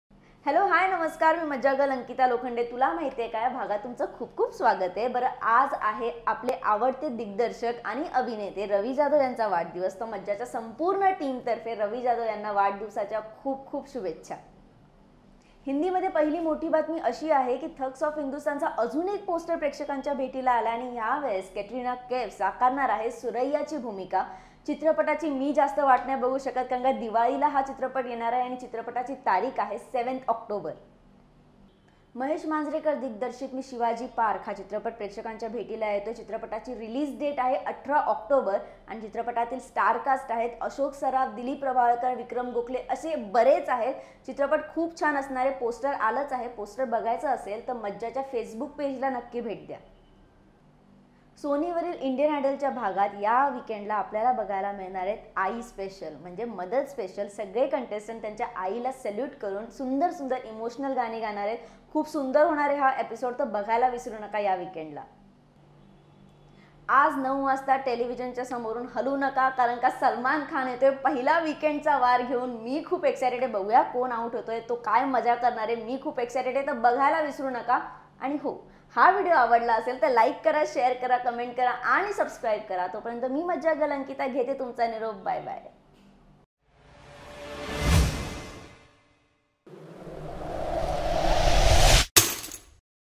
Tula Mahitiye Ka: Bigg Boss 12, Salman Khan, Katrina Kaif, Me Shivaji Park, Ravi Jhadav, And More
Ankita Lokhande is the face of ItsMajja. She is fun, she is vivacious and she the next big thing to happen!! Tula Mahitiye Ka is a daily capsule that is going to a one-stop shop for all entertainment news.

In today's episode get to know about Bigg Boss 12, Salman Khan, Katrina Kaif, Me Shivaji Park, Mahesh Manjarekar, Dilip Prabhavalkar, Vikram Gokhle, Ravi Jhadav, Thugs Of Hindostan, Mahesh Manjrekar, Indian Idol, And More.